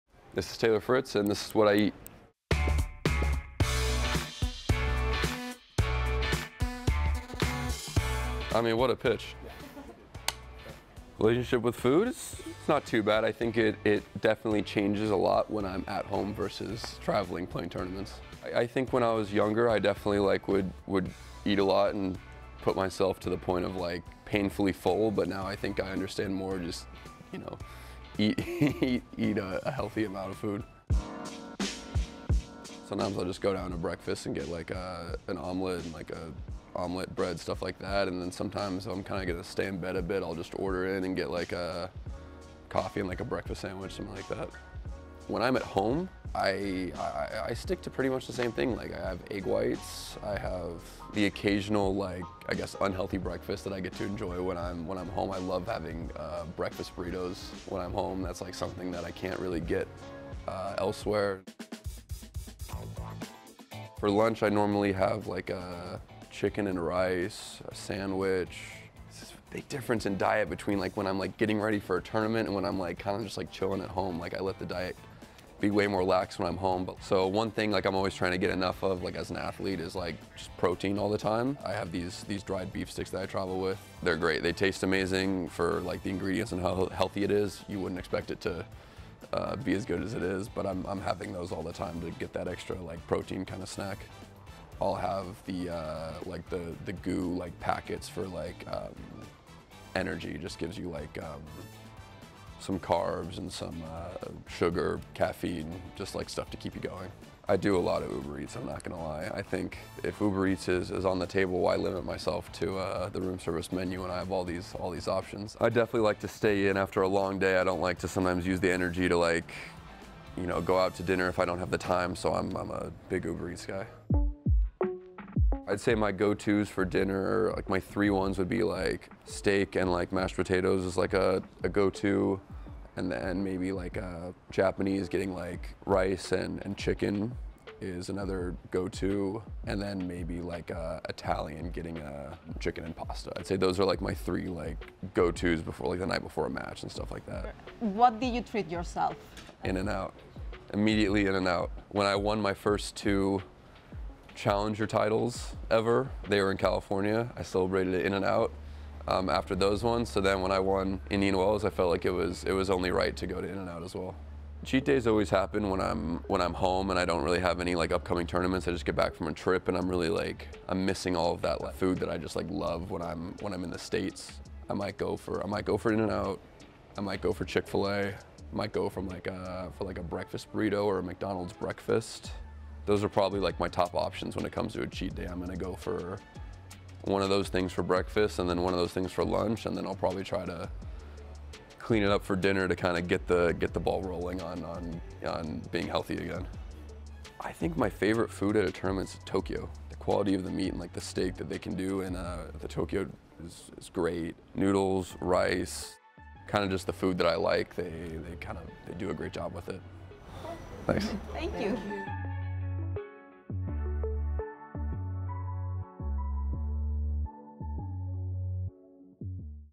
Taylor Fritz reveals his cheat meal and we couldn't agree more! 🍔
What I eat in a day: Taylor Fritz edition 😋